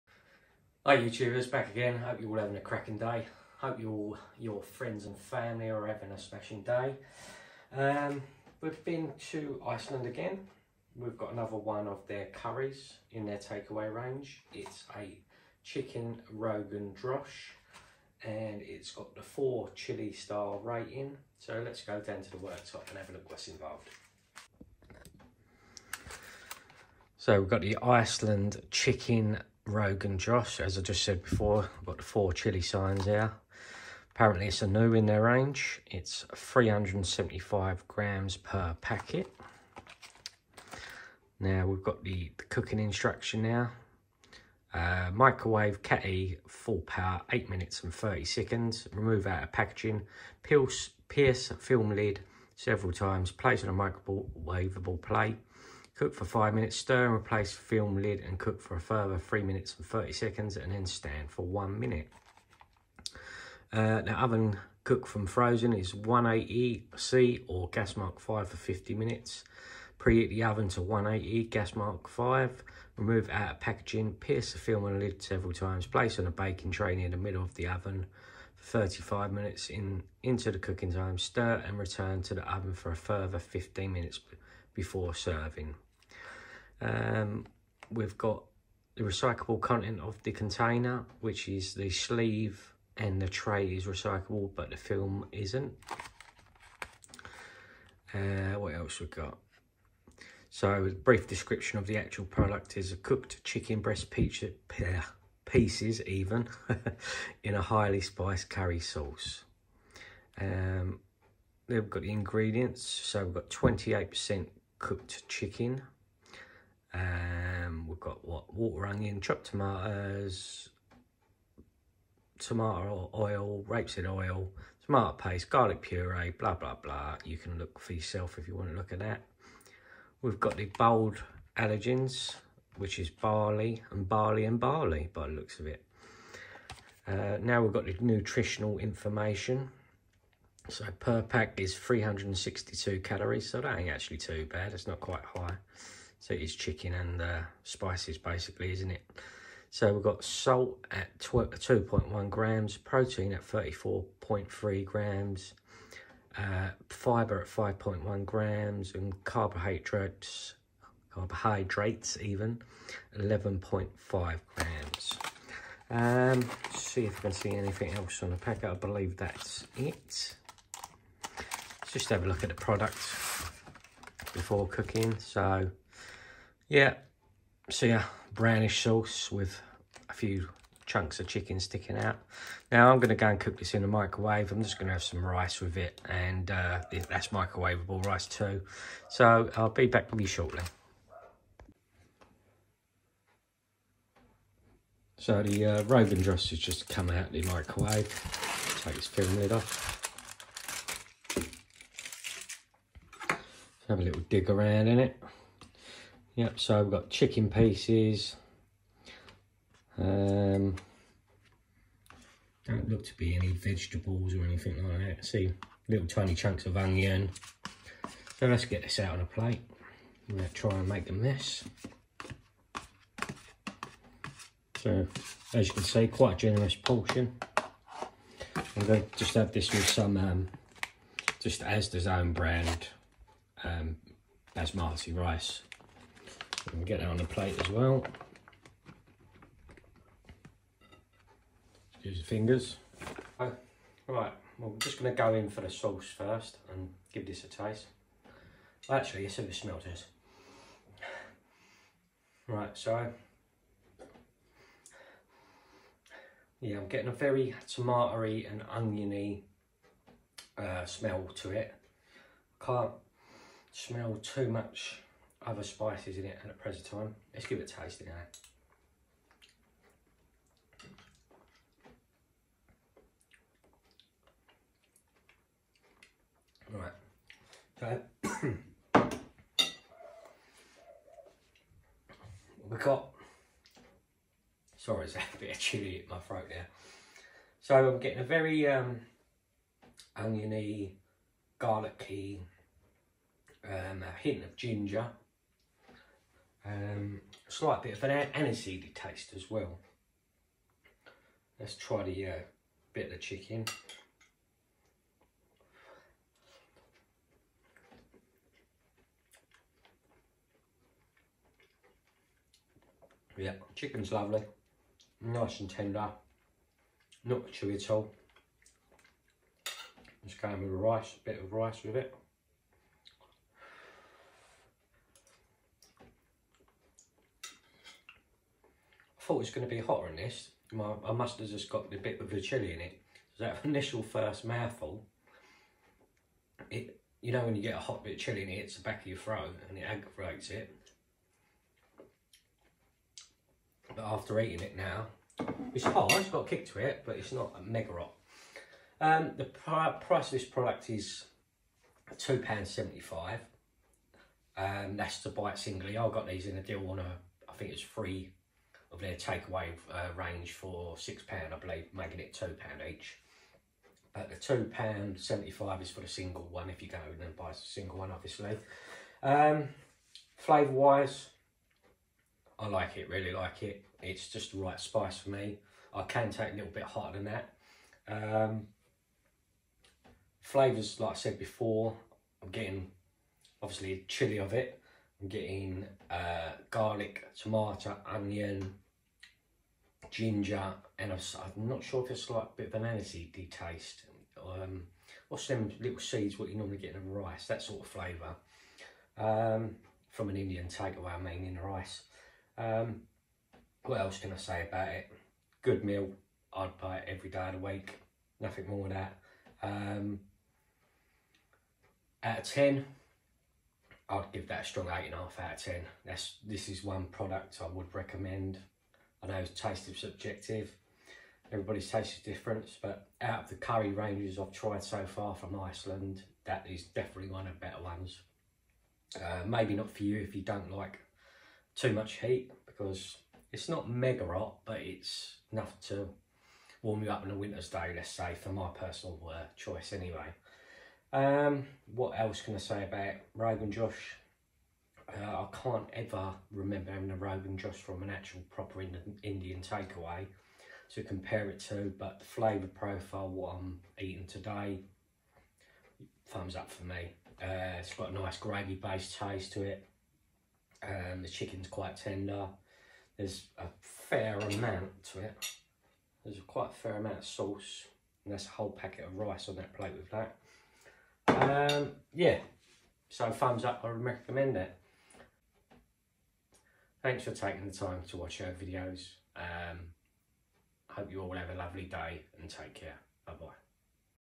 Chicken rogan josh takeaway by Iceland food review
Chicken rogan josh by Iceland spiced chicken curry